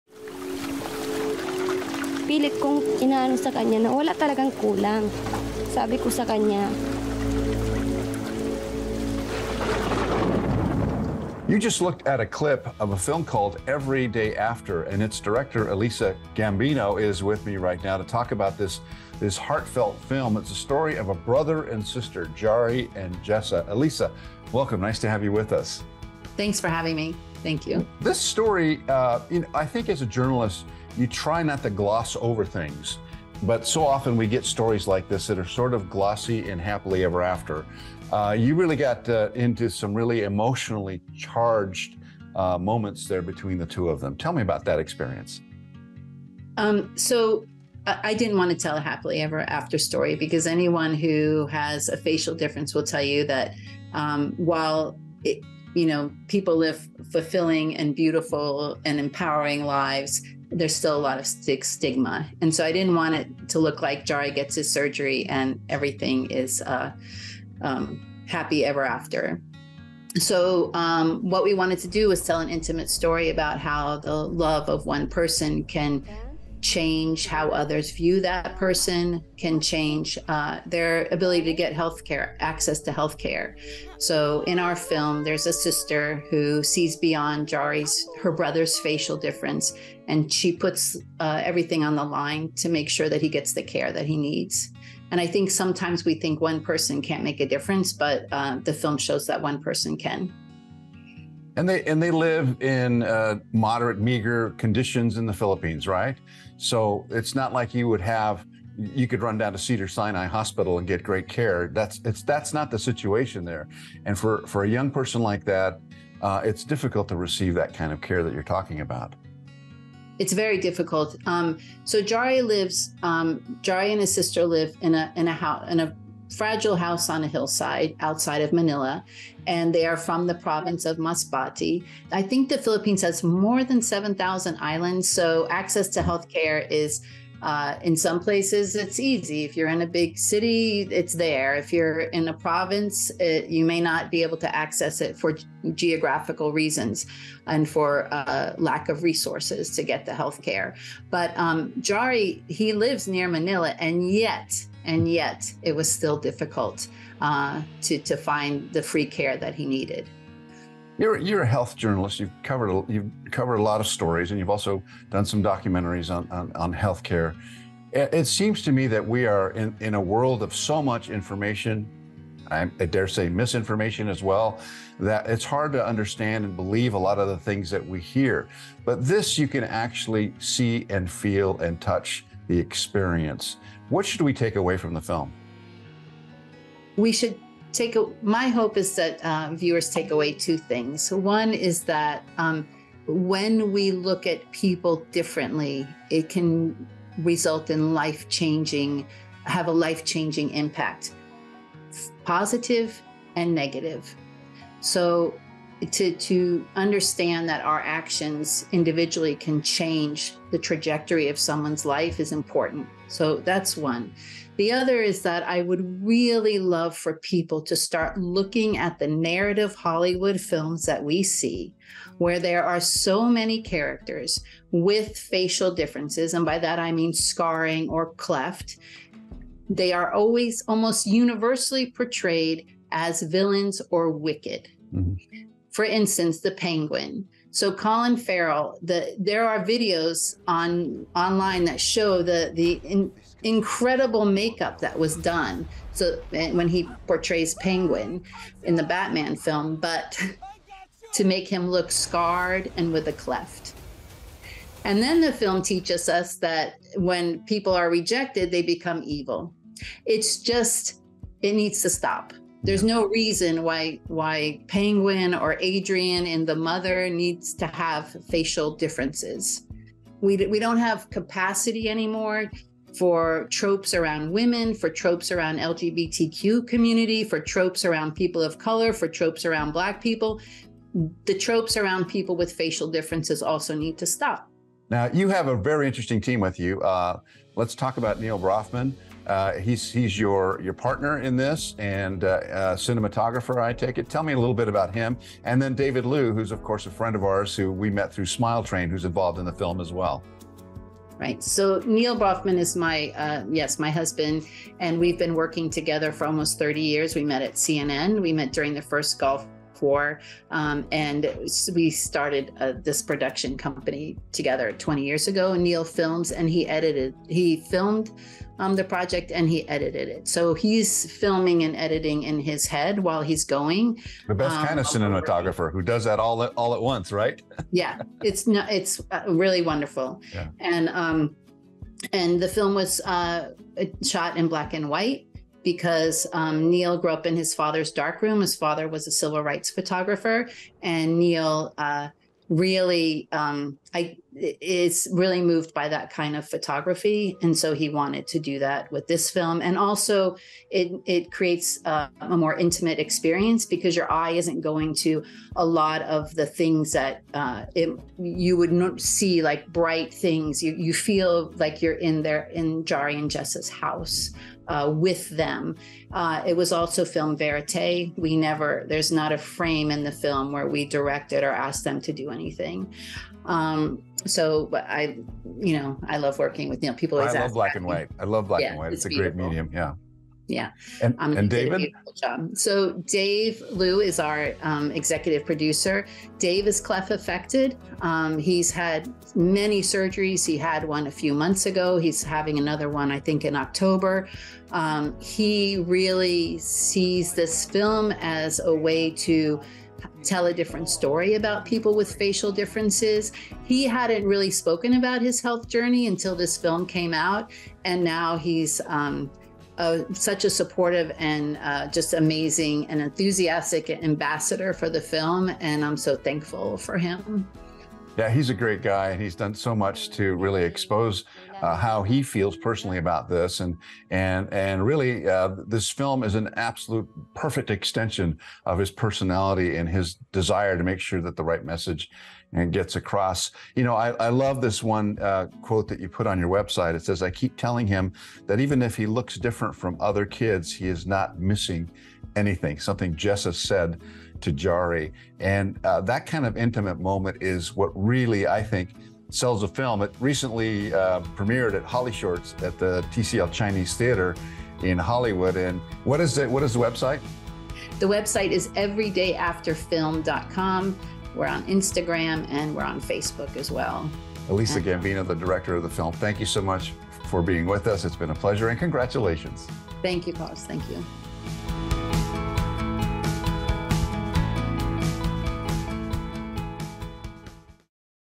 Director Elisa Gambino's "Every Day After" on how one person's love can make a difference
"We wanted to tell an intimate story about how the love of one person can change how others view that person, and can change their ability to get health care, access to health care."
-"Every Day After" Director Elisa Gambino

Elisa Gambino shares the importance behind this short documentary film with Carlos Amezcua. Gambino explains why she didn't want to just tell a happily ever after version of this story and why cleft surgery is more nuanced and emotional than other narratives would have you believe. Plus how she'd like Hollywood to change their storytelling around facial differences and scarring. She uses the Penguin in Batman as an example of why that's so problematic.

Plus Gambino talks about how her filmmaking partners cinematographer Neal Broffman and Executive Producer David Liu helped bring the film to life.

"Every Day After" is premiering next at the Albuquerque Film Festival later this month. You can find more information on their website everydayafterfilm.com

More about the film:

Growing up in Masbate Province in the Philippines, Jary is neglected and shunned since the moment of his birth for one reason-- his appearance.

His older sister, Jessa protects Jary through his early years, then takes him in as a young teen, to raise him alongside her own two children in a fragile house on a hill. Jessa seeks out the medical care Jary has been denied since birth. And more, the support to begin his physical and emotional recovery.

Every Day After is a 35-minute documentary that provides a more nuanced look at the complexities of the healing process we don’t often see. And honors the invisible labor of a sister whose love and action make it possible for Jary to experience the everyday joys and struggles of growing up.

More Carlos & Lisa on BEOND.TV, BEONDTV mobile/smart TV apps and on our global streaming partners.